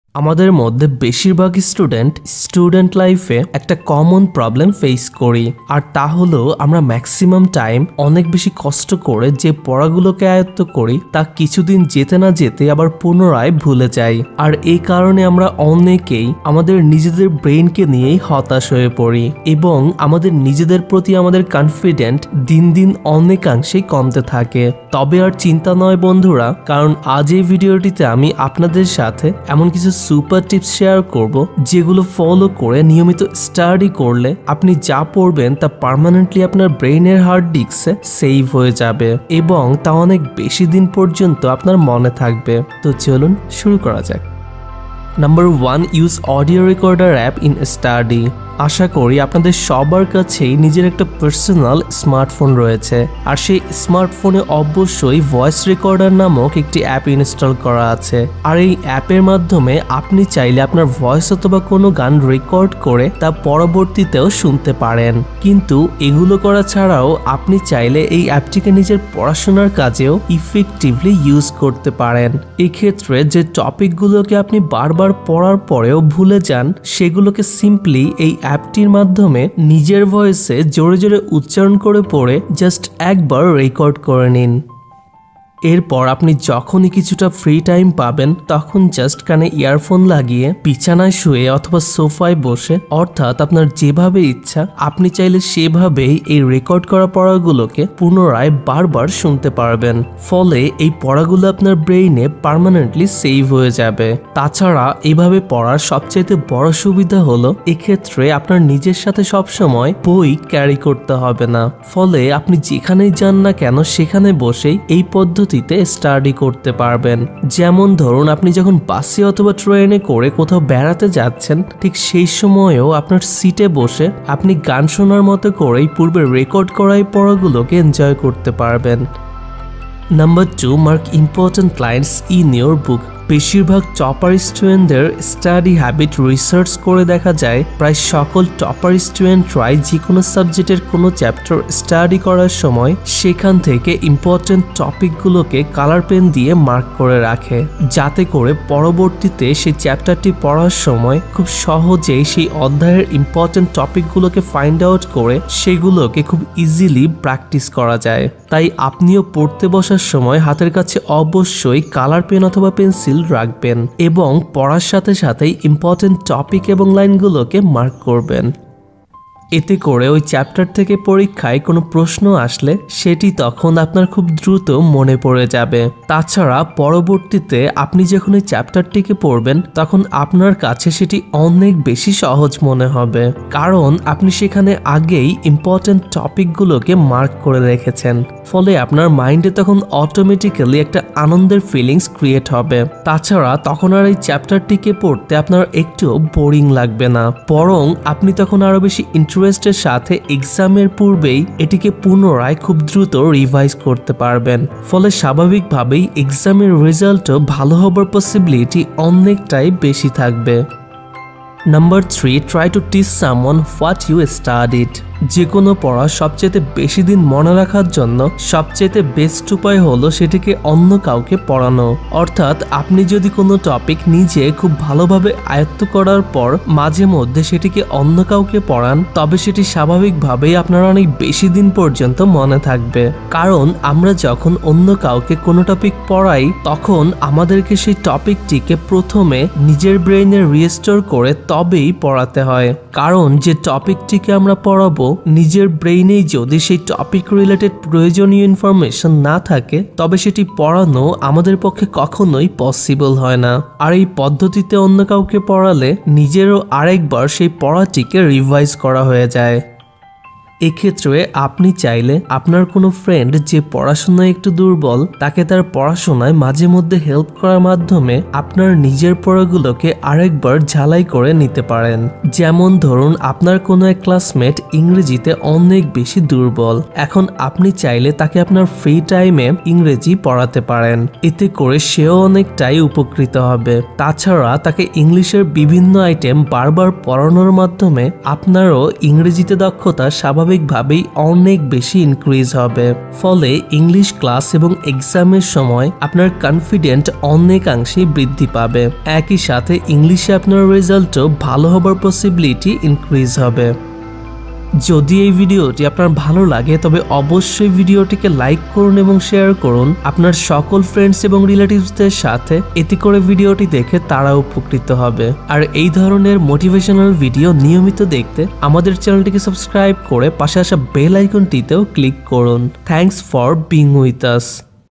মাত্র ১ বার পড়েই যেকোনো পড়া মনে রাখার কৌশল | How to memorize Faster | Study Tips in bangla
Hello Everyone. In this video i have shared with you 3 secret tips to memorize any topic more Faster.

Such as:-
1) Use audio recorder app in study
2) Mark important lines in your book
3) Try to teach someone what you studied

I hope this video will be very helpful for you to study effectively.